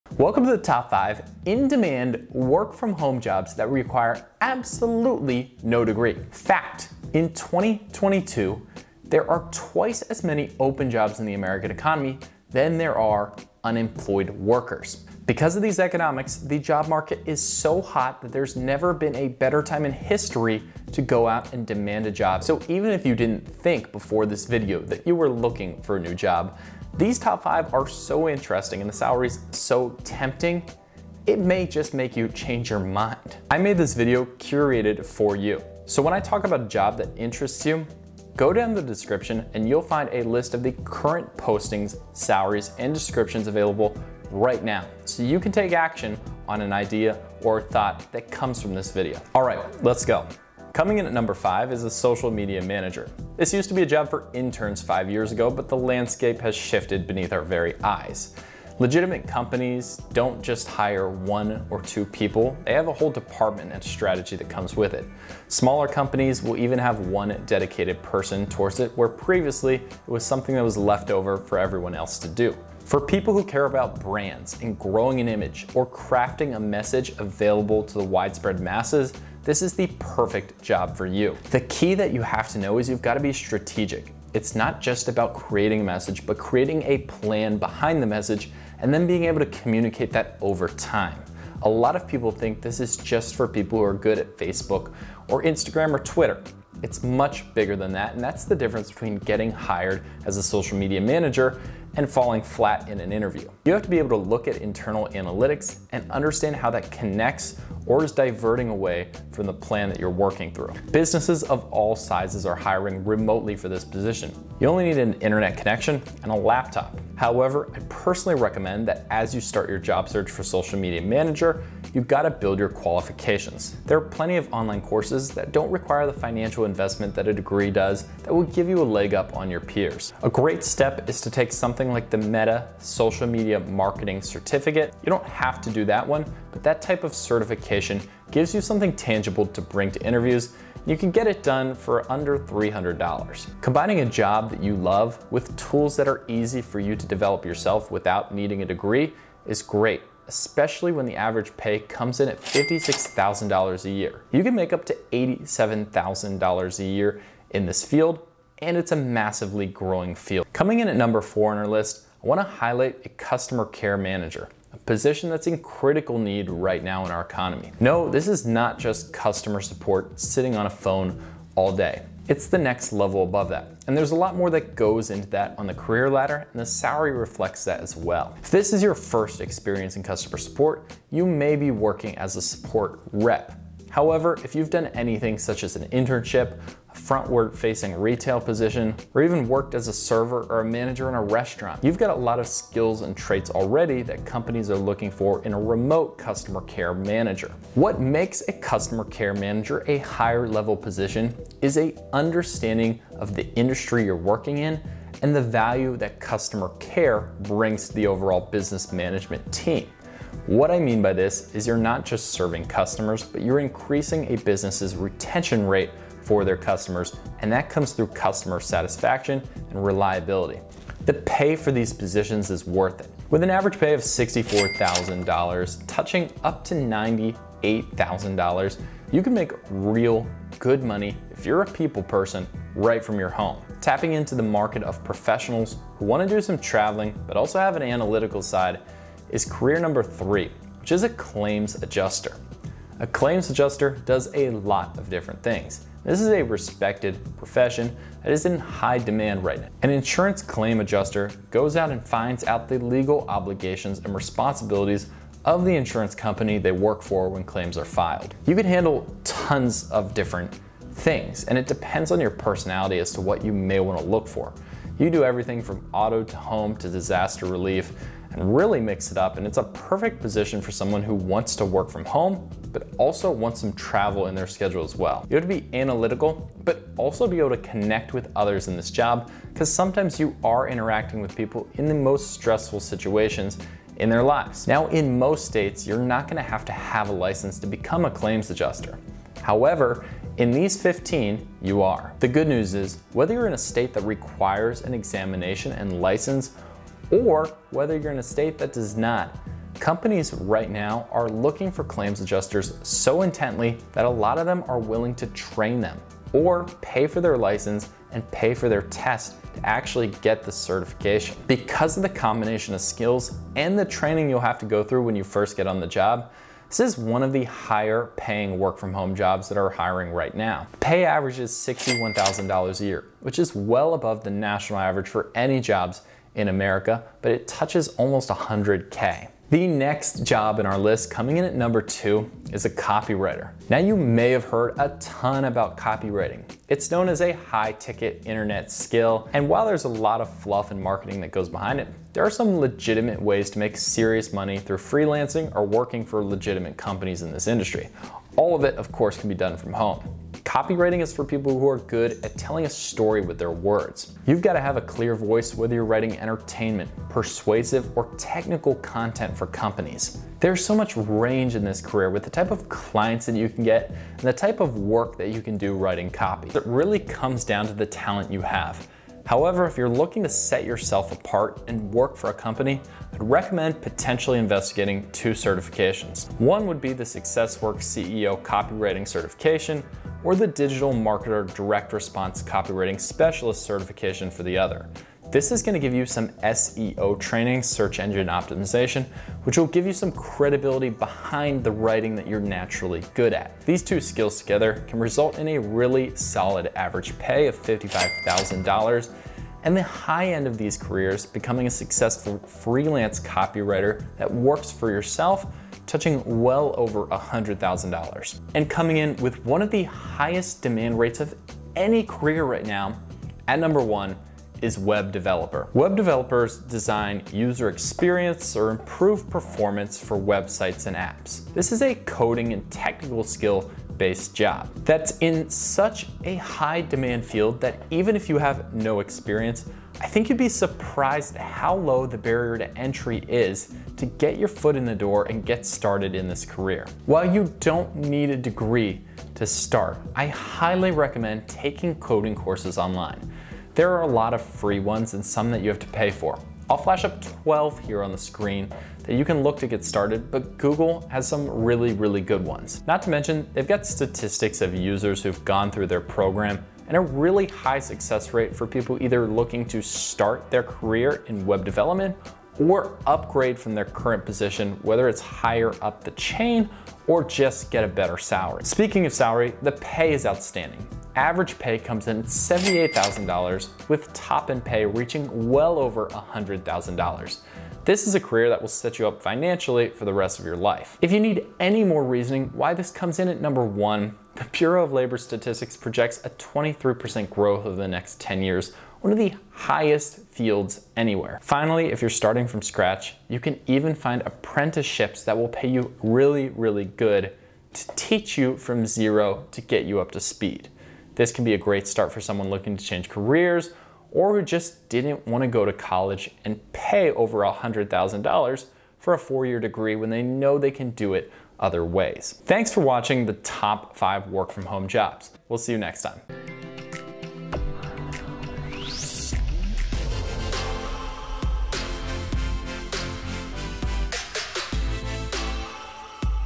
The Top 5 Work-From-Home Jobs with NO College Degree
Welcome to the newest rankings of the most In-Demand, Work-From-Home jobs  that do not require a college degree.

✏️ If you're interested in any of the jobs/careers, I put together a results list of the postings for each in the top 5:

Again, all of these jobs require minimal experience and training. Between the top five, there are:

1. These are interesting jobs that cover a variety of skill sets
2. The salaries on these jobs are way above the median salary in the US. There are two on this list that top earners can consistently make above $100,000.

Top 5 Work-From-Home Jobs with NO College Degree

I also try to give helpful hints on how to get ahead of other applicants for these positions. Some of these involve researching certificates that give you specialized education in the job you're most interested in. Here are the referenced certificates in this video (incase you want to learn more):

Copywriter -
Successworks CEO Copywriting Program

Web Design -

Timestamps:
Intro: 0:00-0:49
Social Media Manager: 0:50-2:40
Customer Care Manager: 2:41-3:54
Claims Adjuster: 3:55-5:30
Copywriter: 5:31-6:48
Web Developer: 6:49-8:47